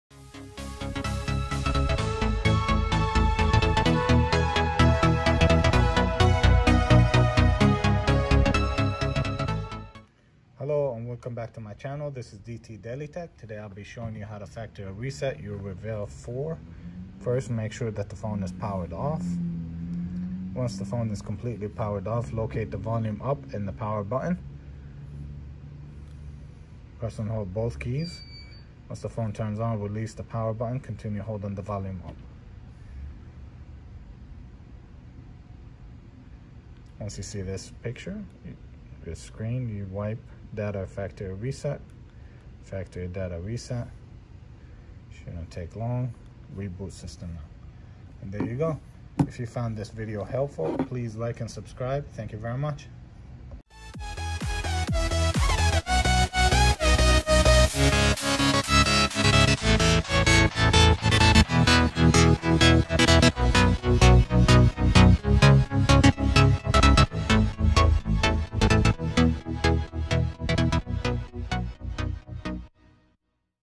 Hard reset T-Mobile Revvl 4 | DT DailyTech
In this video i will be showing you how to factory reset your T-Mobile Revvl 4 to factory settings in less than 5 minutes